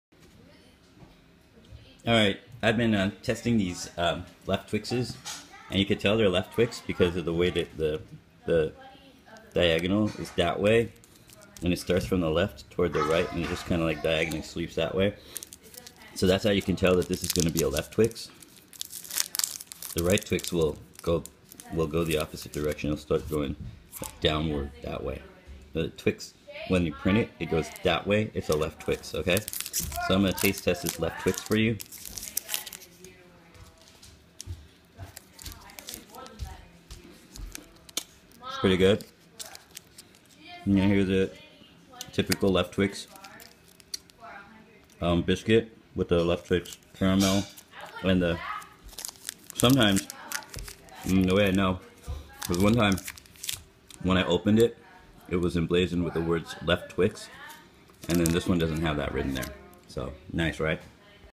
The left Twix and how to tell it is legit
There are a couple of ways to find out if you are eating a left or a right Twix... Find out here.